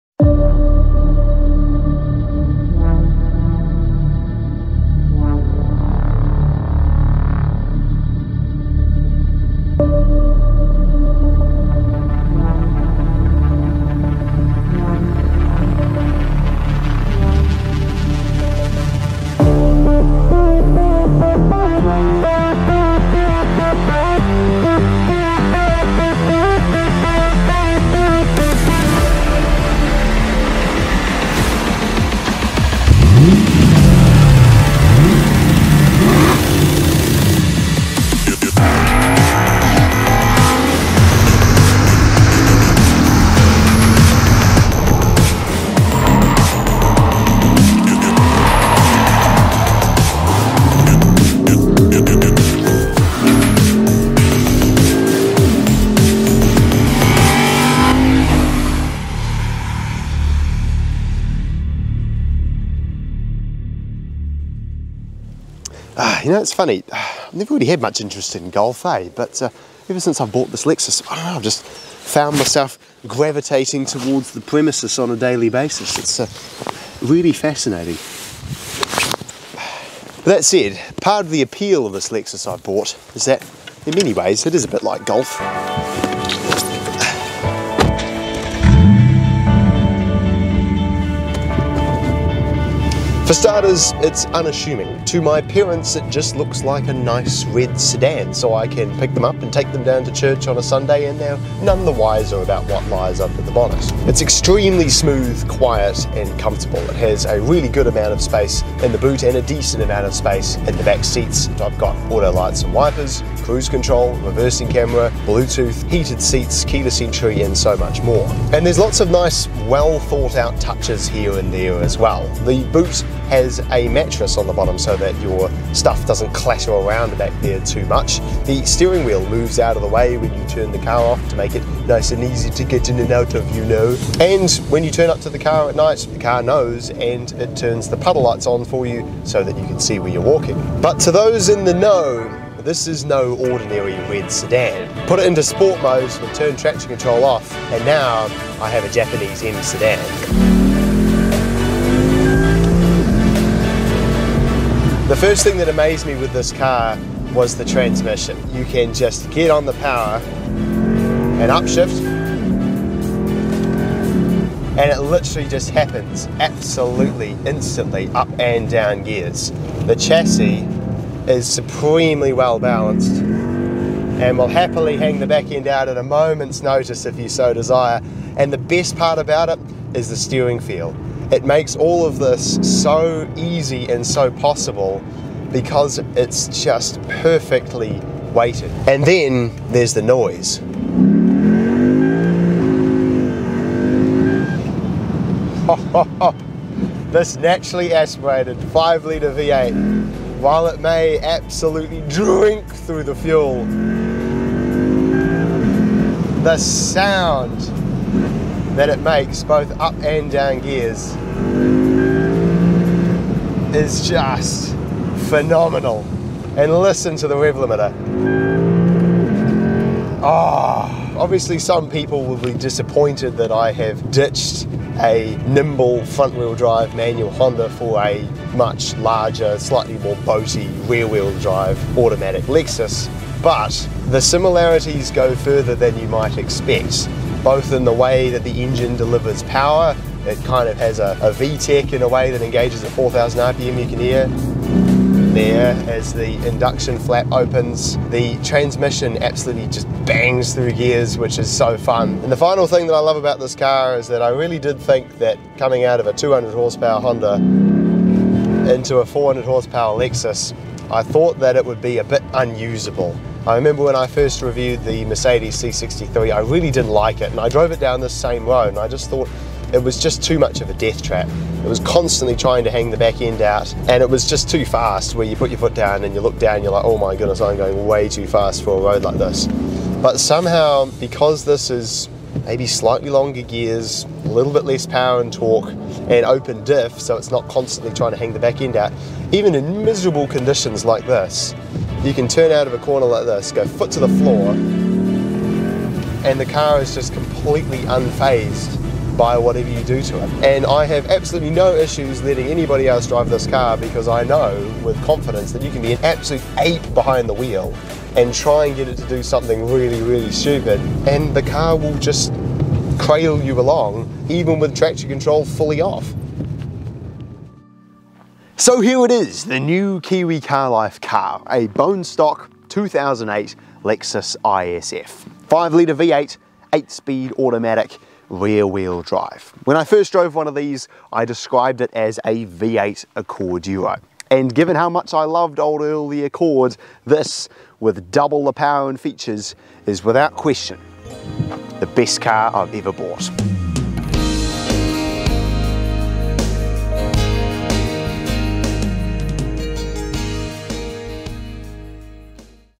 This is the BEST Car I've Bought
BEHOLD my new purchase! A 2008 Lexus ISF. 5.0L V8, 8 speed auto, RWD. What more could a man want.